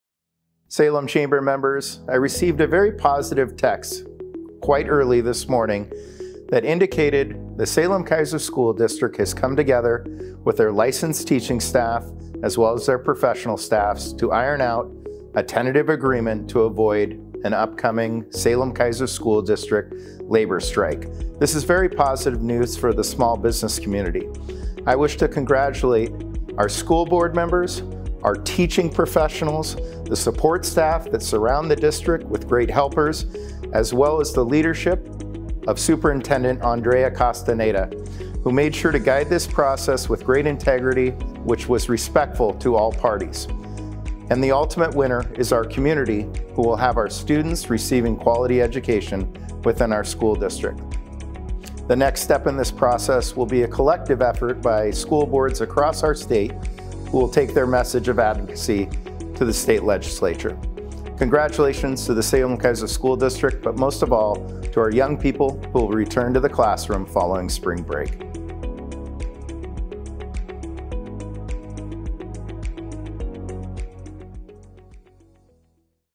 SKPS Reaches Tentative Agreement with Education Association to Avoid Strike
Salem Chamber CEO Tom Hoffert shares the positive news that the Salem-Keizer School District has reached a tentative agreement with the Salem-Keizer Education Association, meaning a teacher's strike will be avoided, schools will remain open, and students will return to class following Spring Break.

On behalf of students, families, and the business community, we thank all those involved for finding a resolution to these labor negotiations and avoiding a potentially harmful strike.